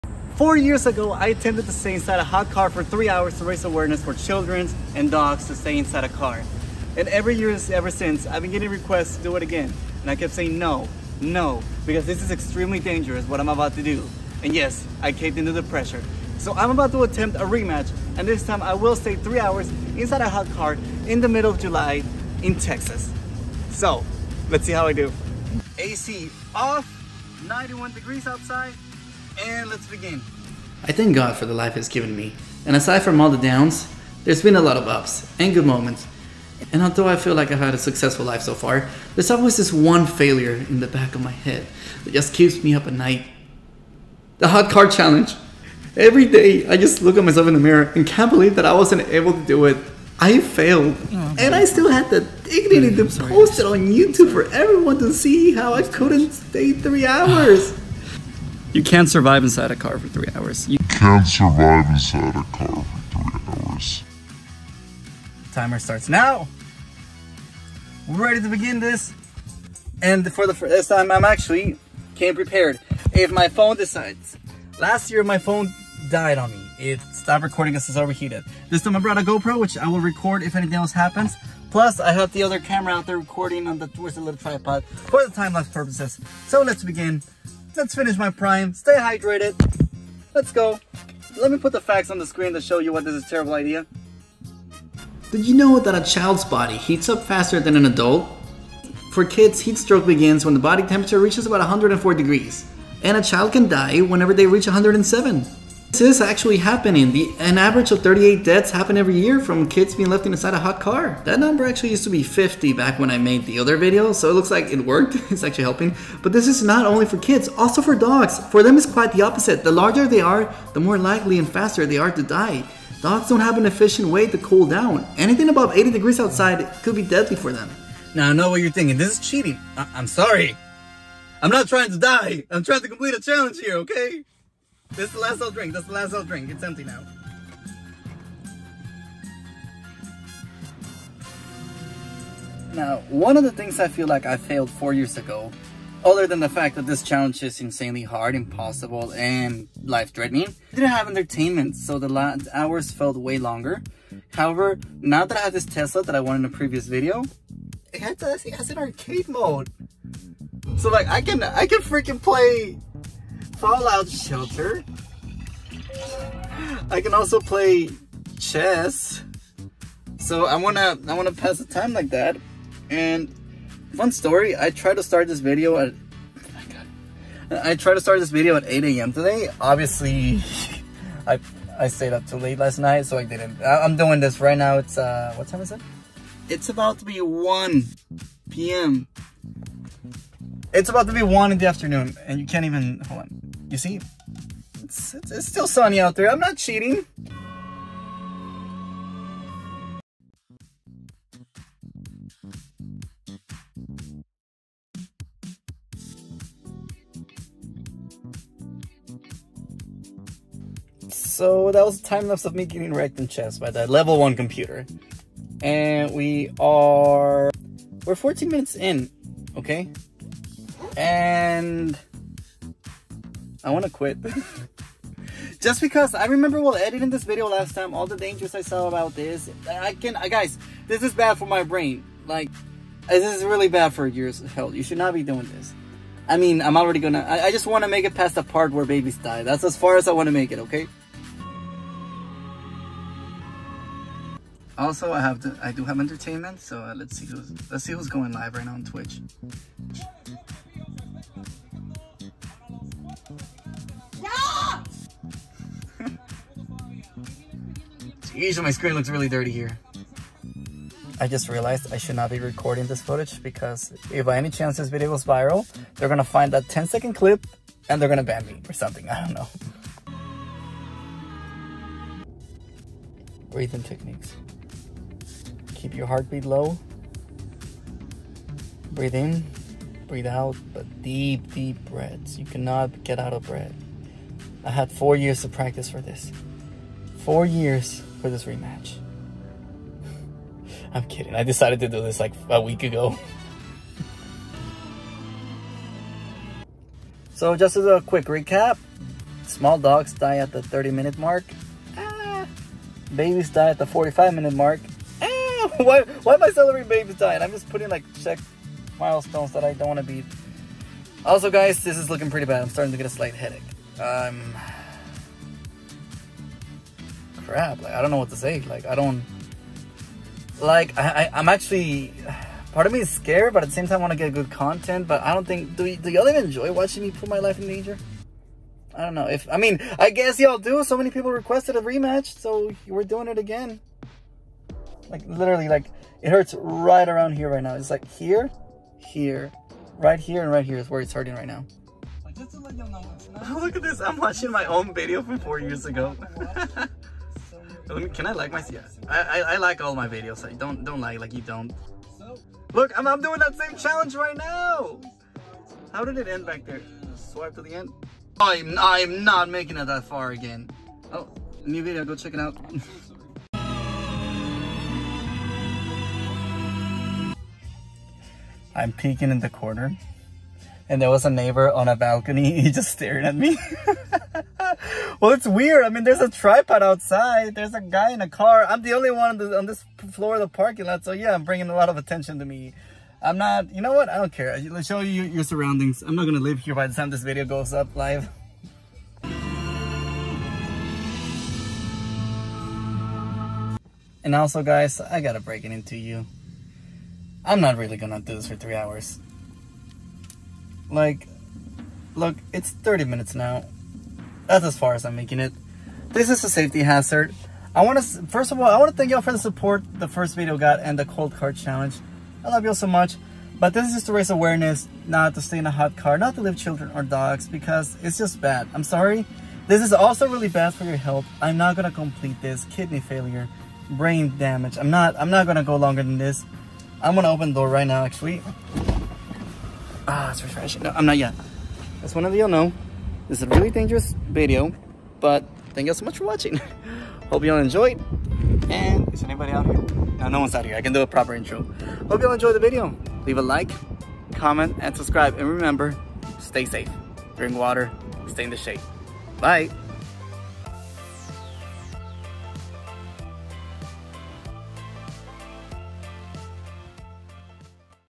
I Spend 3 Hours Inside a Hot Car (Challenge Rematch)
This has been my most request video ever, and after denying to do it for the past 4 years I finally decided to do it again.

If by any chance ya'll would like to add me and I'll add you back, I'm on